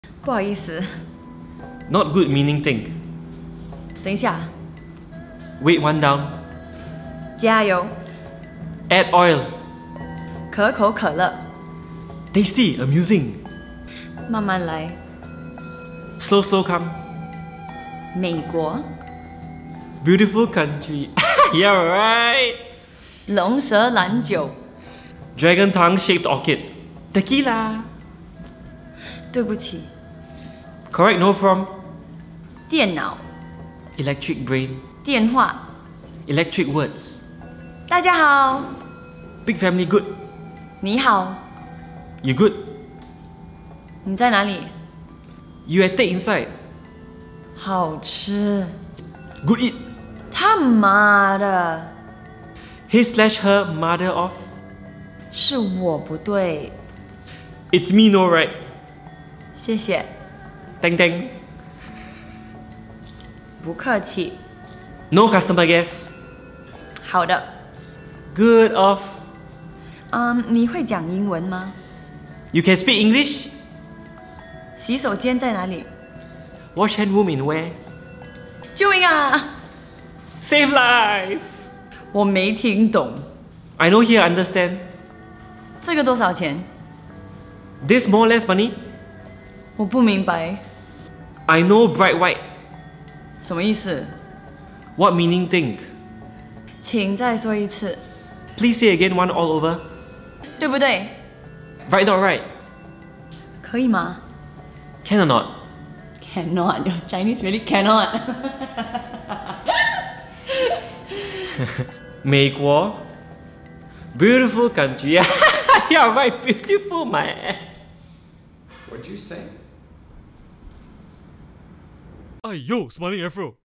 Mandarin Direct To English
If mandarin was directly translated to english... this might be what we hear.